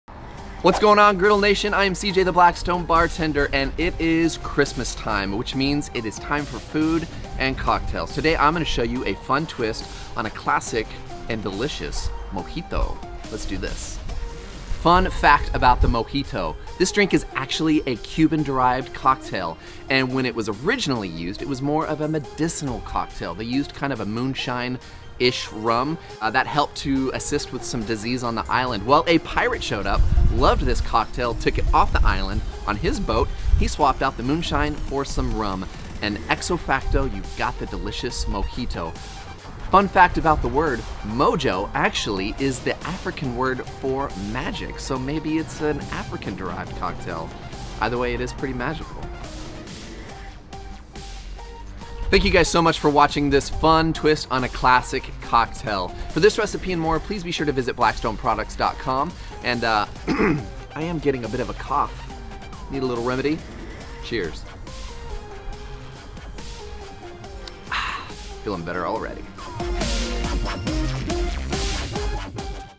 Christmas Coconut Mojito | Blackstone Griddle
Cj, The Blackstone Bartender, has a special Holiday Cocktail treat for you! In this video, he will show you how to make a fun Christmas twist on the Classic Mojito. No matter what you have fired up on the Blackstone this holiday season, be sure to pair it with a delicious cocktail... your guests will thank you!